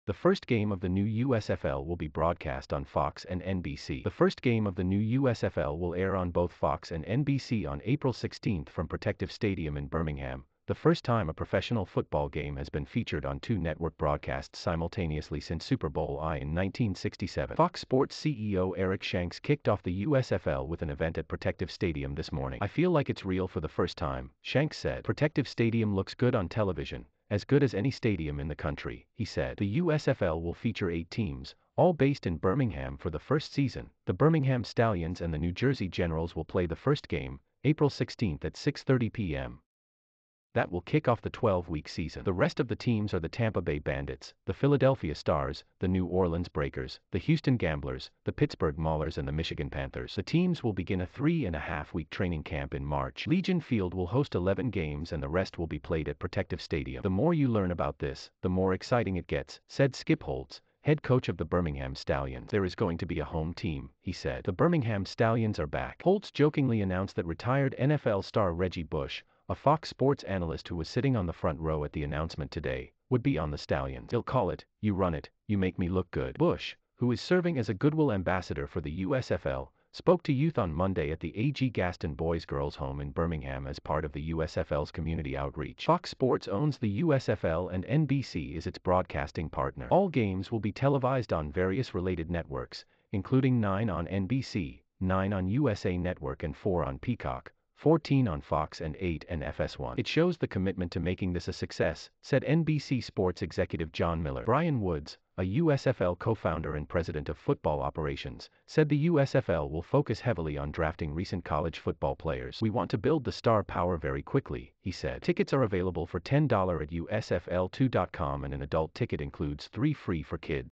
The first game of the new USFL will be broadcast on Fox and NBC
The first game of the new USFL will be broadcast on Fox and NBC The first game of the new USFL will air on both FOX and NBC on April 16 from Protective Stadium in Birmingham he said. The USFL will feature eight teams The Birmingham Stallions are back. The Birmingham Stallions and the New Jersey Generals will play the first game the first time a professional football game has been featured on two network broadcasts simultaneously since Super Bowl I in 1967. Fox Sports CEO Eric Shanks kicked off the USFL with an event at Protective Stadium this morning. a USFL co-founder and president of football operations Shanks said. Protective Stadium the Pittsburgh Maulers and the Michigan Panthers. The teams will begin a three and a half week training camp in March. Legion Field will host 11 games and the rest will be played at Protective Stadium.